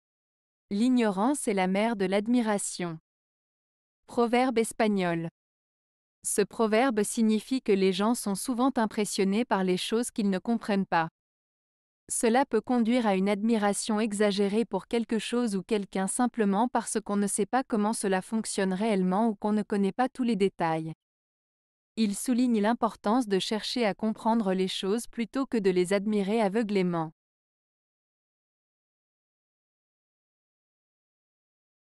L'ignorance est la mère de l'admiration. - Proverbe Espagnol
Ce proverbe signifie que les gens sont souvent impressionnés par les choses qu'ils ne comprennent pas. Cela peut conduire à une admiration exagérée pour quelque chose ou quelqu'un simplement parce qu'on ne sait pas comment cela fonctionne réellement ou qu'on ne connait pas tous les détails. Il souligne l'importance de chercher à comprendre les choses plutôt que de les admirer aveuglément.